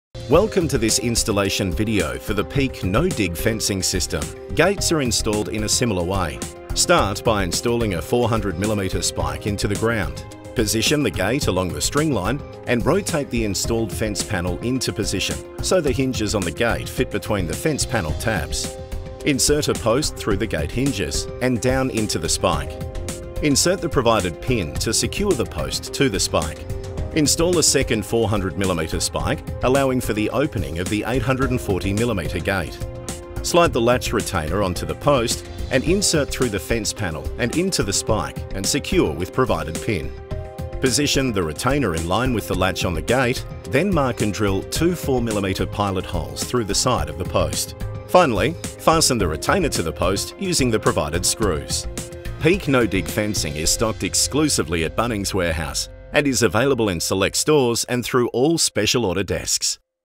Peak No Dig Fencing Gate Installation
Welcome to this installation video for the PEAK No-Dig Fencing system.

Peak No-Dig Fencing is a durable, galvanized and powder-coated steel fencing system that installs in minutes, with no digging required, and is available exclusively at Bunnings Warehouse.

To begin the installation process, first measure your area to determine the type and quantity of components you will need.
Then, ensure you have all the tools and safety equipment for your project.
It is recommended to start your fence installation at an end, corner or gate.
Mark the starting position with a wooden stake. Then use a string line to layout the location of your fence, and to ensure it is aligned.
Layout the fence panels all facing the same direction for a consistent look with the pickets on the same side of the rail.
Place a 400mm spike at the marked end and drive it partially into the ground. Ensure the spike is plumb using a post and level then continue until the top of the blades are flush with the ground.
Use a wood block to prevent damage to the finish.
Hold your first fence panel in position and insert the post through the fence panel tabs and down into the spike.
Align the pre-drilled holes in the post and spike, then insert the provided pin to secure the parts together.
Using the string line as a guide, rotate the fence panel into position, then insert a post down through the tabs on the opposite side of the fence panel and press it into the ground.
This will mark the position for your next spike.
Remove the post.
Rotate the panel to the side while you install the second spike into the marked position.
Do this in the same way as the first and ensure it is plumb, and flush with the ground.
Position the second fence panel on the string line and align it with the first.
Insert the post through the fence panel tabs, connecting them together, and down into the spike.
Insert the provided pin to secure the post to the spike.
Repeat these installation steps for the remainder of fence panels in your project.
Gates are installed in a similar way.
First, determine the direction of swing of your gate
Start by installing a 400mm spike into the ground.
Position the gate along the string line and rotate the installed fence panel into position so the hinges on the gate fit between the fence panel tabs.
Insert a post through the gate hinges, and down into the spike.
Insert the provided pinto secure the post to the spike.
Install a second 400mm spike allowing for the opening of the 840mm gate.
Slide the latch retainer onto the post and insert through the fence panel and into the spike and secure with a provided pin.
Position the retainer in line with the latch on the gate then mark and drill two 4mm pilot holes through the side of the post.
Finally, fasten the retainer to the post using the provided screws.
And there you have it, you’ve successfully installed your Peak No-Dig Fencing system.
Peak No-Dig Fencing is stocked Exclusively at Bunnings Warehouse and is available in select stores and through all Special Order Desks.